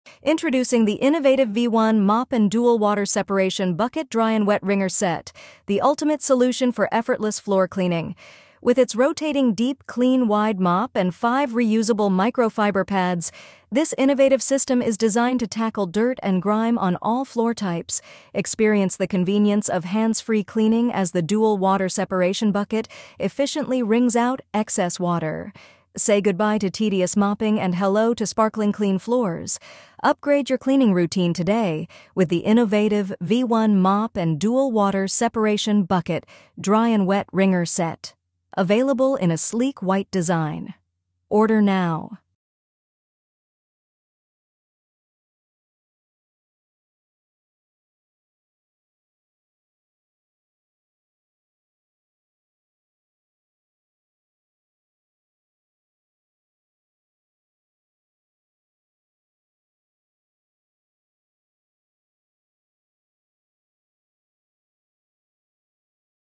Effortless Cleaning Perfected: INNOVATIVE V1 Mop & Dual Water Separation Bucket Set!
Try the INNOVATIVE V1 Mop and Dual Water Separation Bucket! 🧼✨

Clean your floors effortlessly with this advanced mop featuring a dual water separation bucket. Keep dirty and clean water separate for a more effective clean!